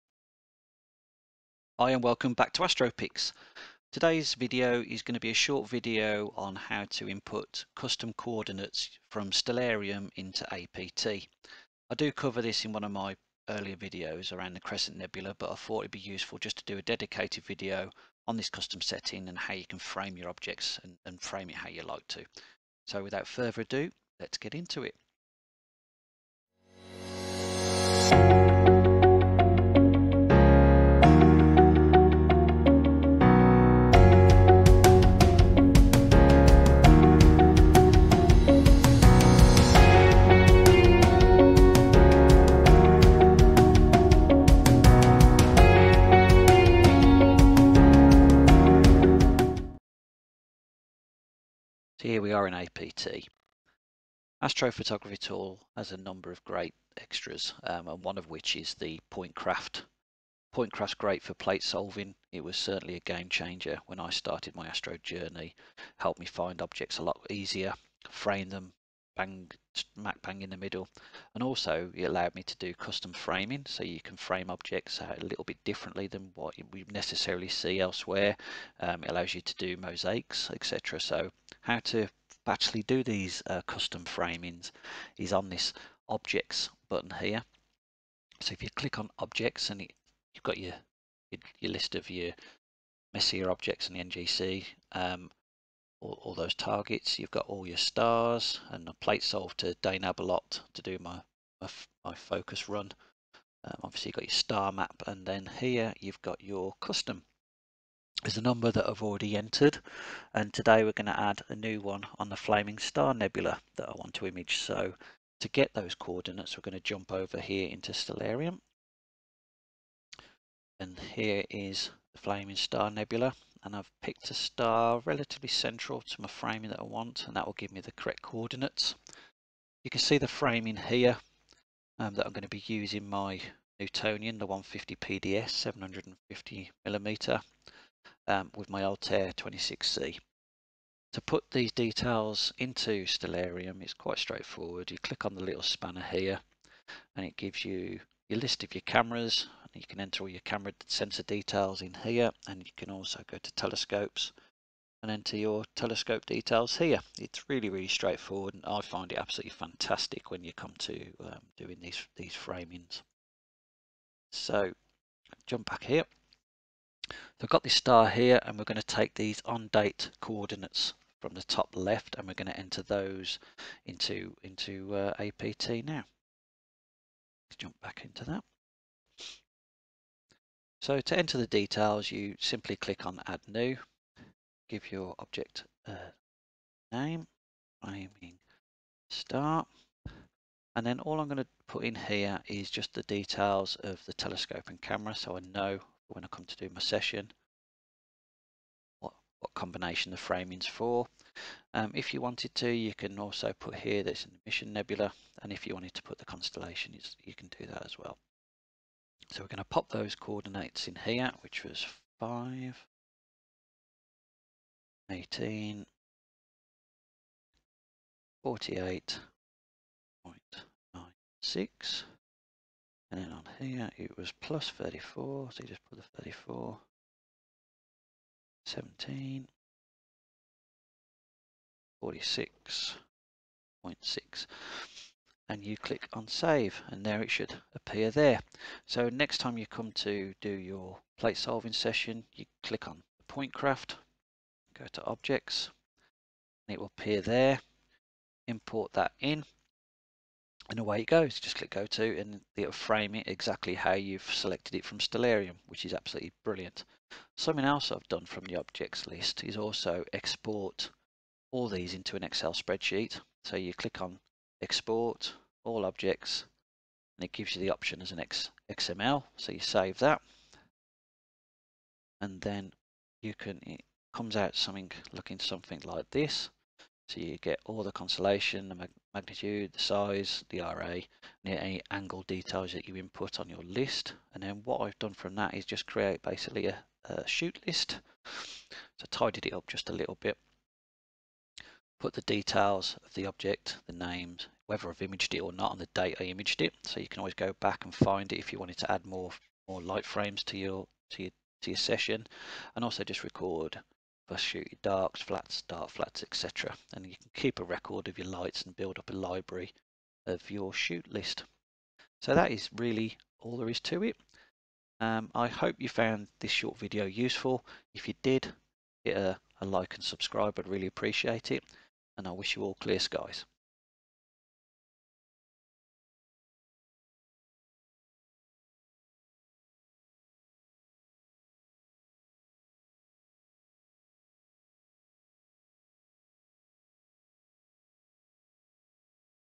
Astro Photography Tool - Custom Coordinates
Video on how to use Coordinates from Stellarium to create a custom shoot list in Astro Photography Tool.